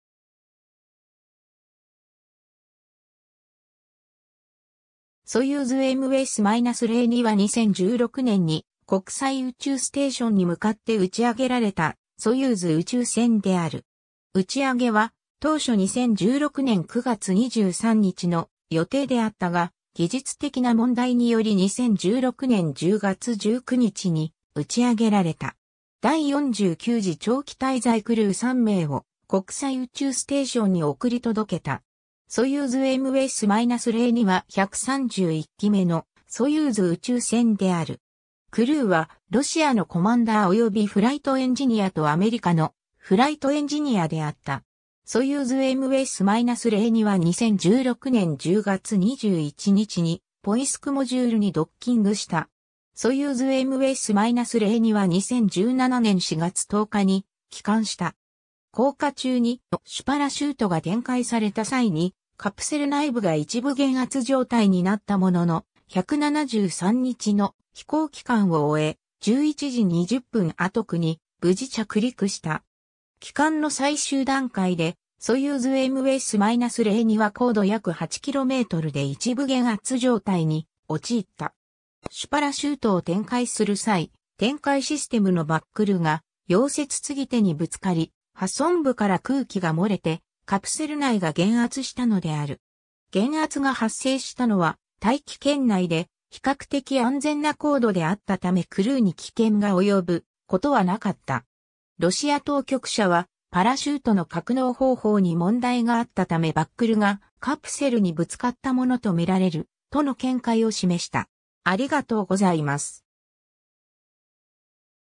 ソユーズMS-02
ソユーズMS-02は2016年に国際宇宙ステーションに向かって打ち上げられたソユーズ宇宙船である。
打ち上げは当初2016年9月23日の予定であったが、技術的な問題により2016年10月19日に打ち上げられた。
第49次長期滞在クルー3名を国際宇宙ステーションに送り届けた。
ソユーズMS-02は131機目のソユーズ宇宙船である。
クルーはロシアのコマンダーおよびフライトエンジニアとアメリカのフライトエンジニアであった。
ソユーズMS-02は2016年10月21日にポイスクモジュールにドッキングした。
 ソユーズMS-02は2017年4月10日に帰還した。
降下中に主パラシュートが展開された際にカプセル内部が一部減圧状態になったものの、173日の飛行期間を終え、11:20 UTCに無事着陸した。
 帰還の最終段階で、ソユーズMS-02は高度約8キロメートルで一部減圧状態に陥った。
主パラシュートを展開する際、展開システムのバックルが溶接継手にぶつかり、破損部から空気が漏れてカプセル内が減圧したのである。
減圧が発生したのは大気圏内で、比較的安全な高度であったためクルーに危険が及ぶことはなかった。
ロシア当局者は、パラシュートの格納方法に問題があったためバックルがカプセルにぶつかったものとみられる、との見解を示した。